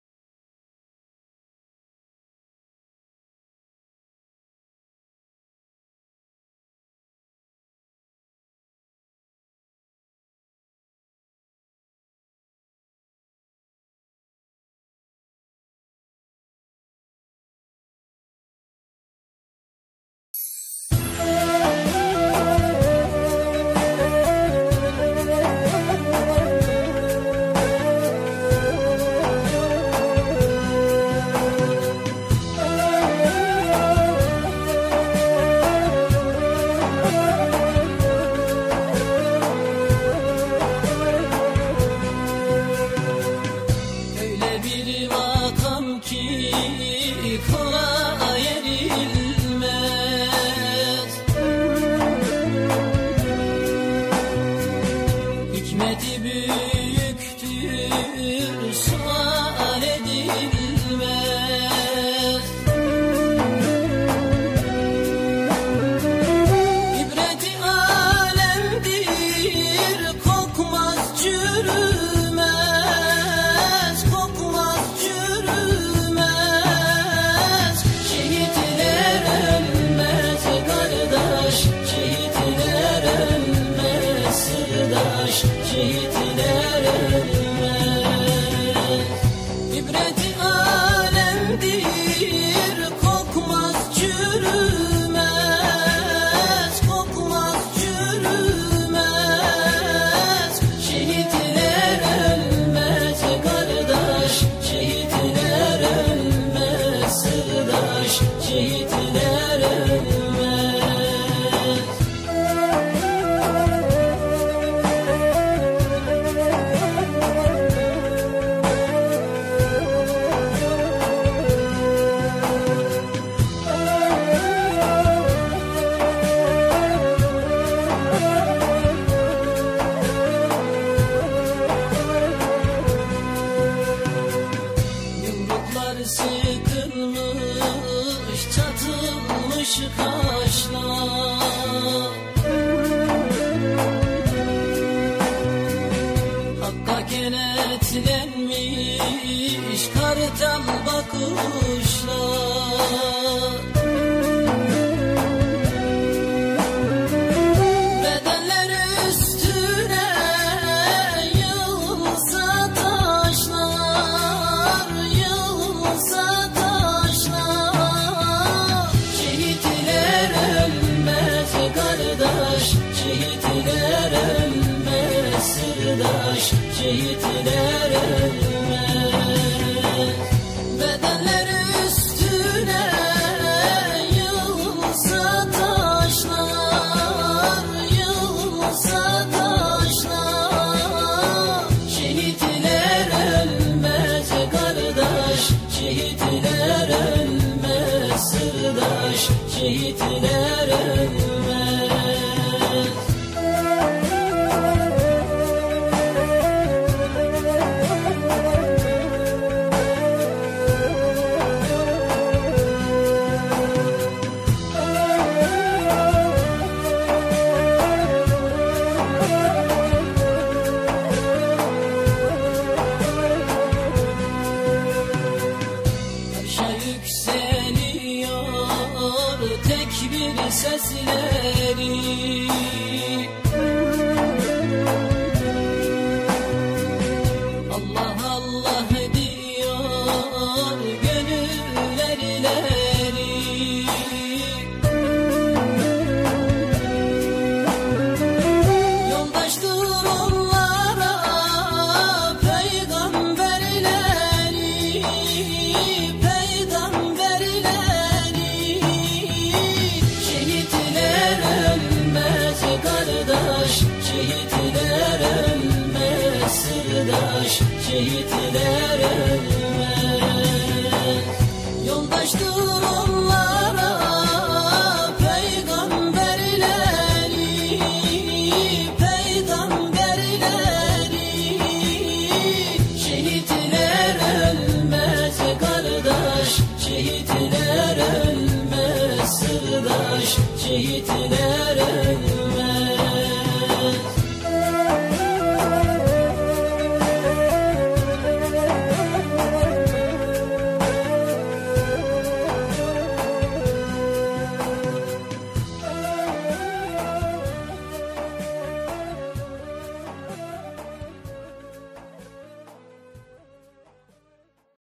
ŞEHİT MEHMET POLAT'I ANMA MERASİMİ
Şehidimiz Mehmet Polat'ı şehadetinin ikinci yılında mezarı başında andık. Allah rahmet eylesin.